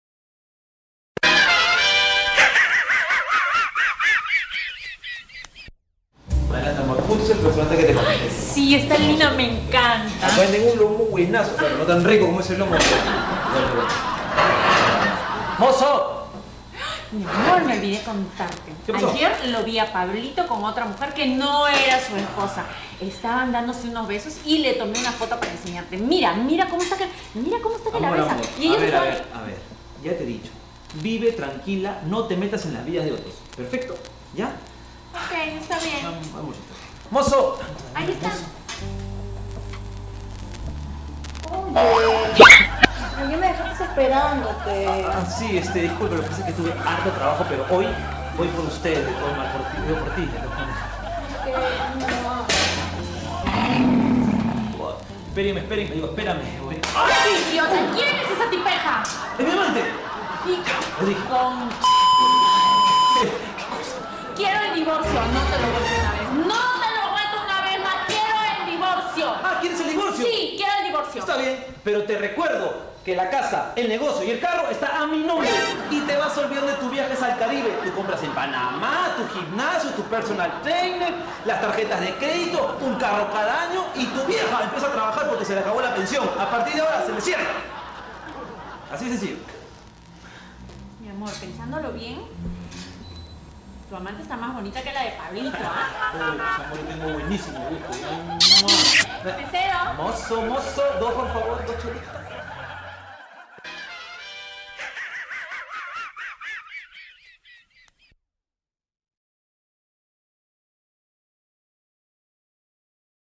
CHESU TV Web - Una Mujer Comprensiva
Aqui otro corto cómico más para CHESU TV Web. Con Canelita Amoretti y Omar Claveri, espero les guste :) .Cámara y Edición: John Arturo Obregón Hervias